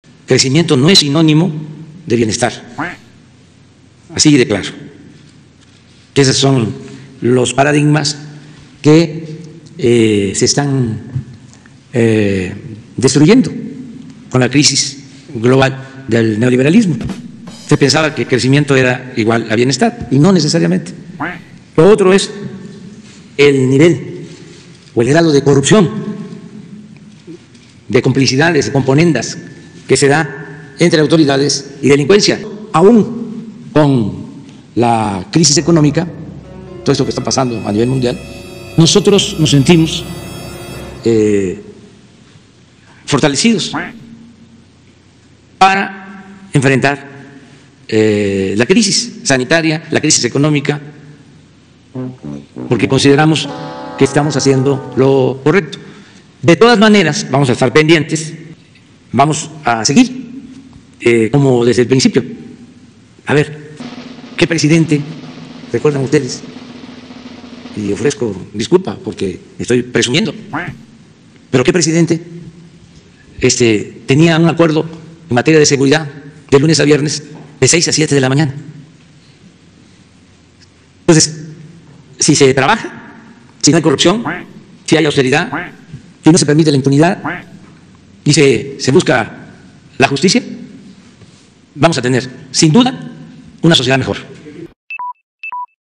“Crecimiento no es sinónimo de bienestar”-AMLO
“Crecimiento no es sinónimo de bienestar”- @lopezobrador_
Como ya no hay corrupción, estamos fortalecidos para enfrentar la crisis sanitaria y económica. Y no va cambiar su ‘estrategia’ 🦆